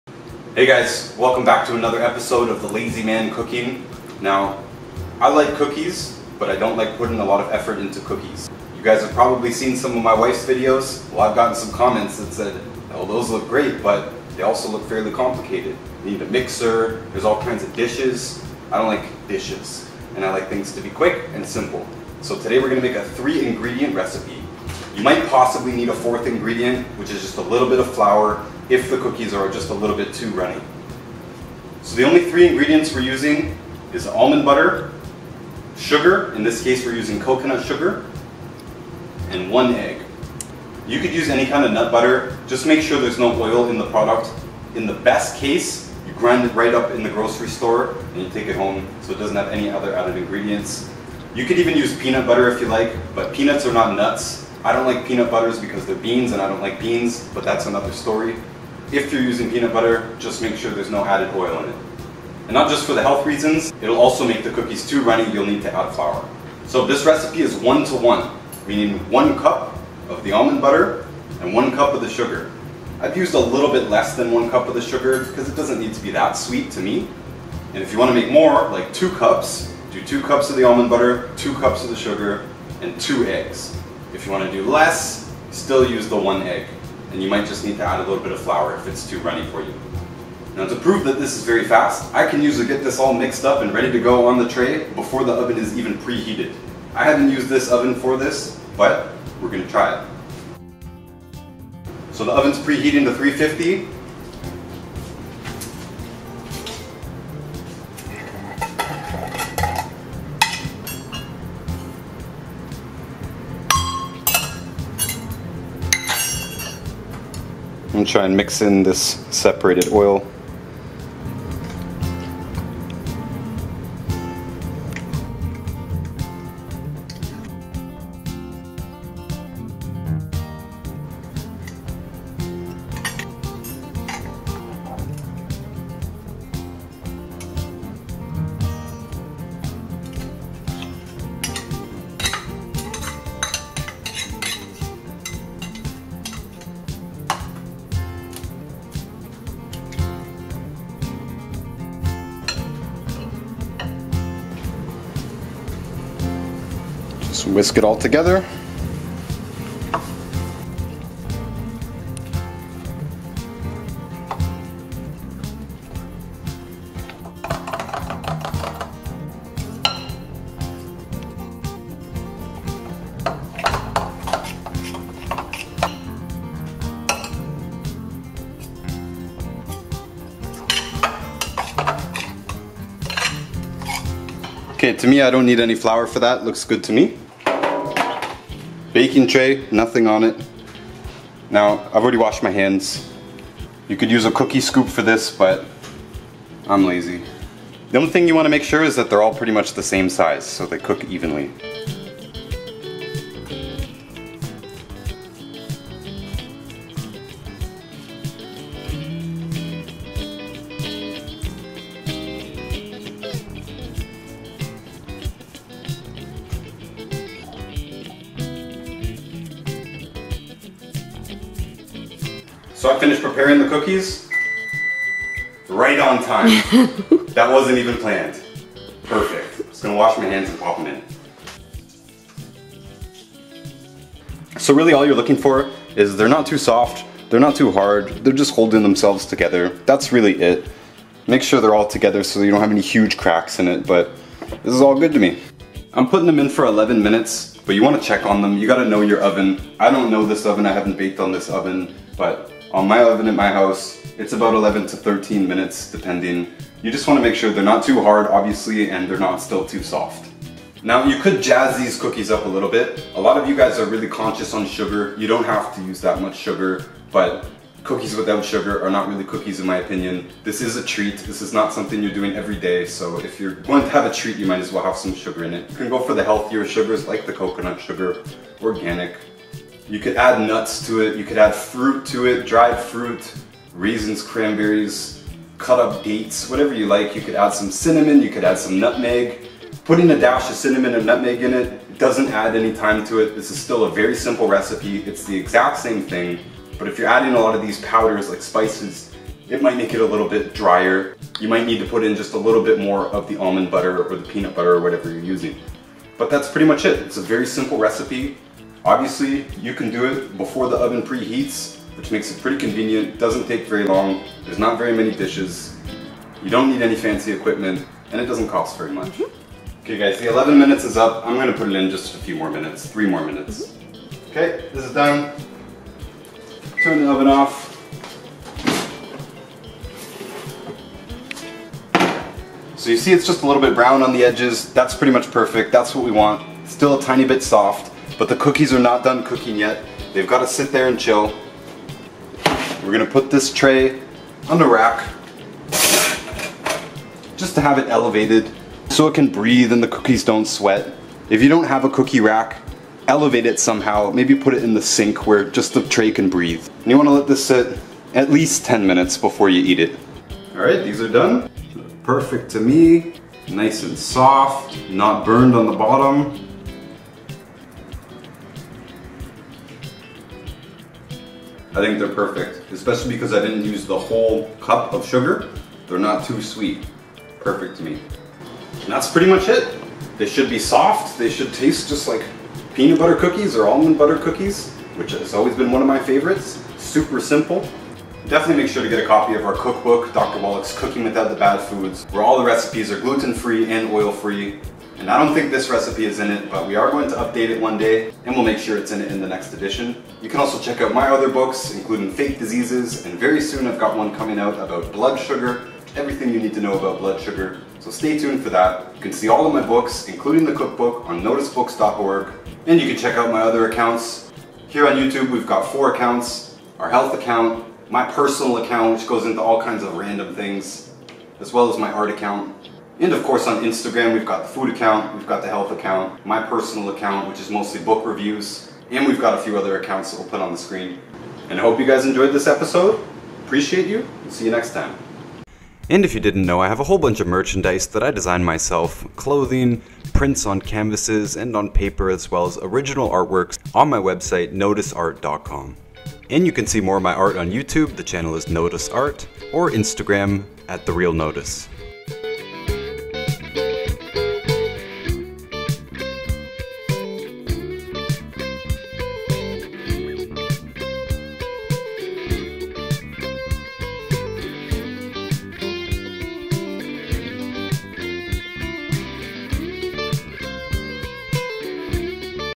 Simplest 3 Ingredient Cookies - Gluten-Free - Lazy Man Cooking
Recipe:
1 cup nut butter
1 cup sugar (or a bit less)
1 egg

you might need some gluten-free flour.

mix ingredients together, bake at 350F 11-15 mins (depending on your oven), until slightly brown and crispy on the edges.

let cookies rest another 10 mins out of the oven before serving.

optional additions: nuts, dry fruits, cinnamon, other spices.

Dr. Wallach's Cooking Without The Bad Foods (complete cookbook)